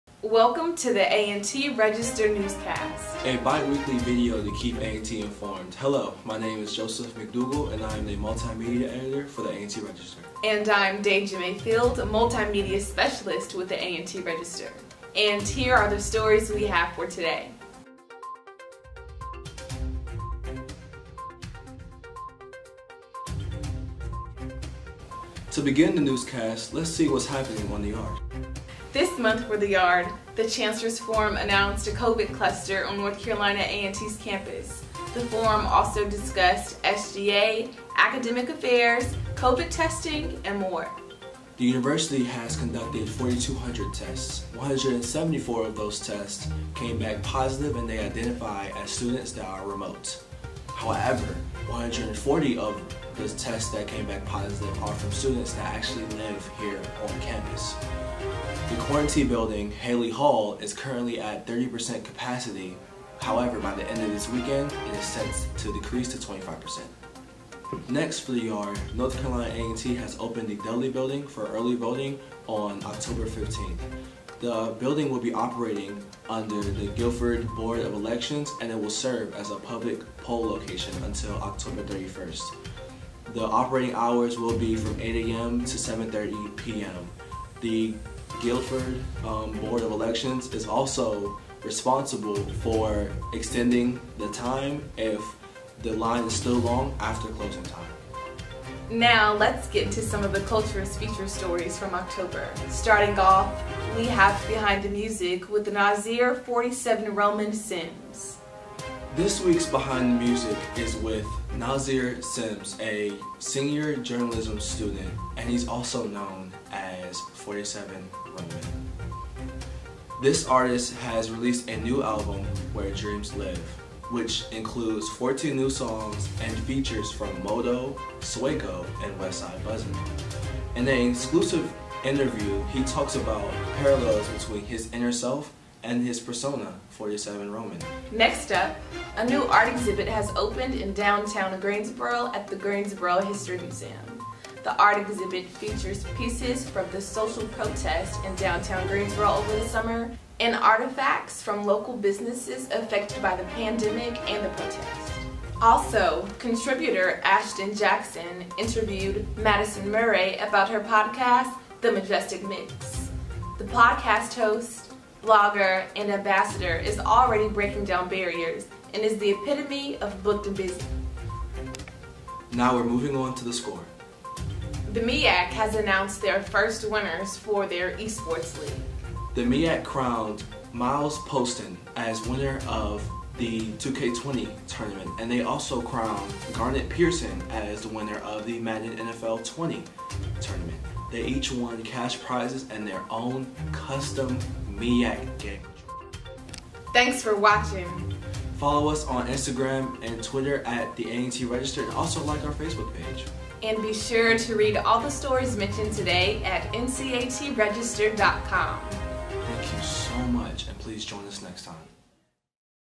The A&T Register Newscast 10-18-20
The A&T Register Newscast is a bi-weekly update video to keep A&T students informed.

On this newscast, see what you missed in the news.

Shot by: Jaylin Saunders, Digital Media Editor
Edited by: Josef McDougal, Multimedia Editor